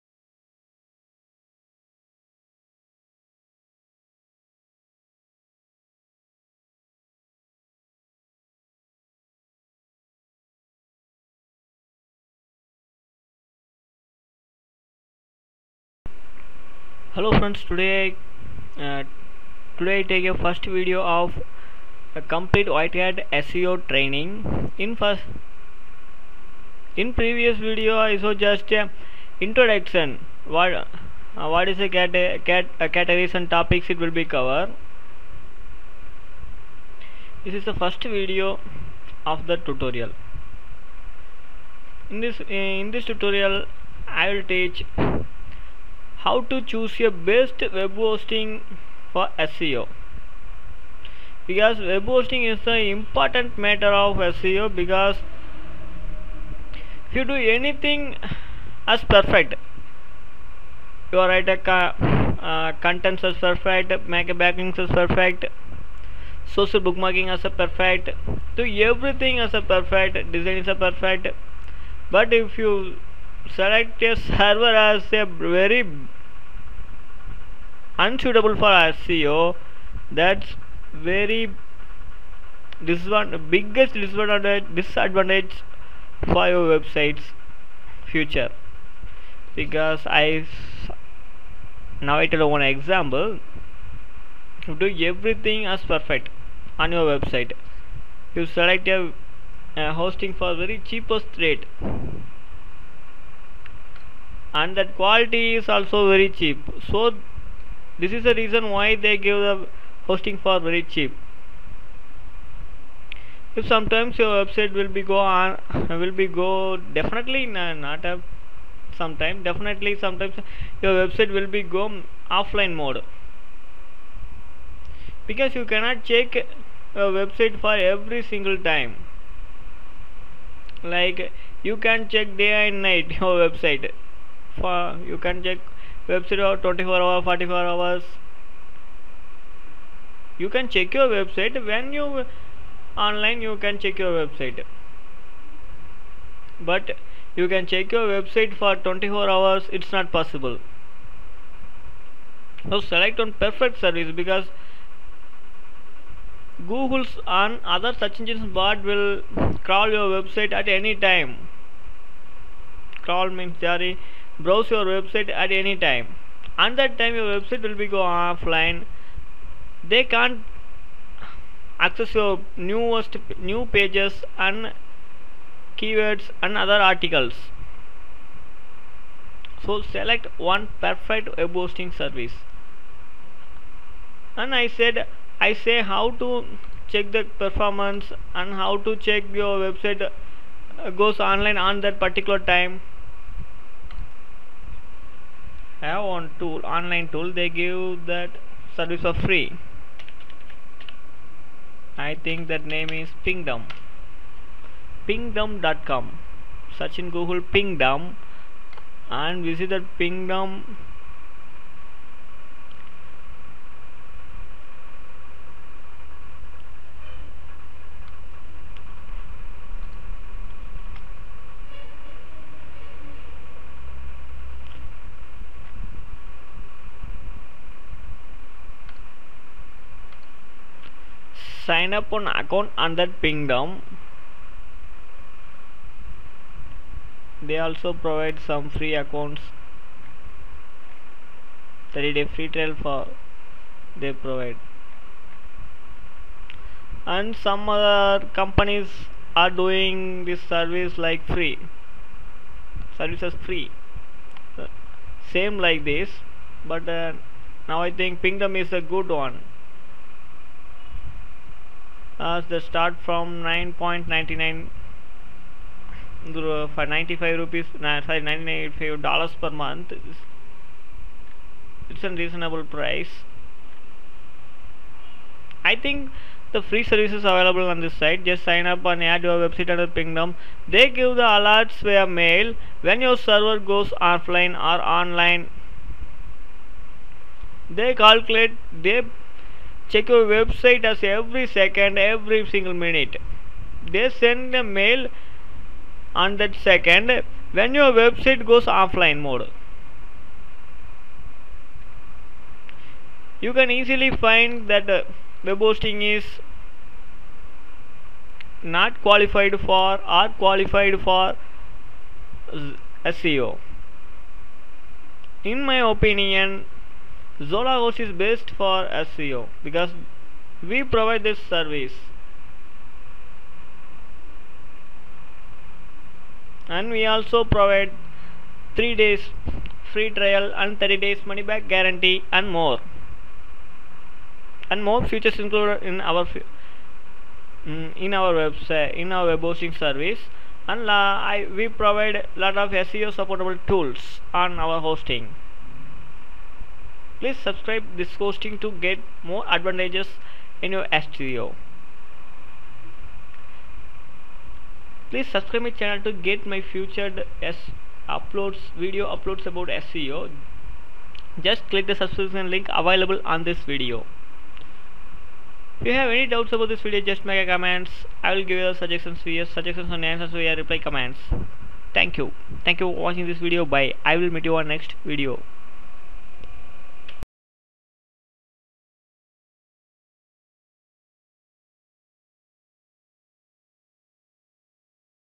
Suitable Web Hosting Service For Do SEO
Hello friends,

In previous tutorial just gave the introduction. In this tutorial, I start a first video of complete white hat SEO training. You need to choose a best web hosting service suitable for SEO. Because of the server performance is very important in Search Engine Optimization. In this tutorial video I told you why you need to select a perfect web hosting plan that is suitable for your site get a higher ranking such engines. If you have any doubts about this video, just make a comment and I'll give you a answer via reply comment. Please send this video with your friends and like this video if this video is really useful for you.